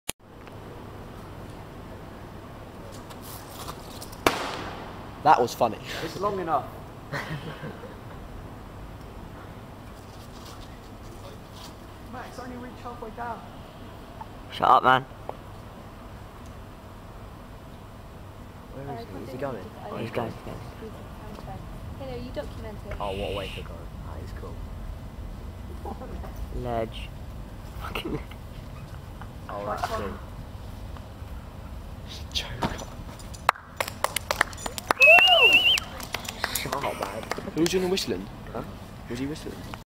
The Bentalls Centre Abseil!
Video of me abseiling down the 6 floors of the bentalls centre car park. Thanks to Luke for filming this!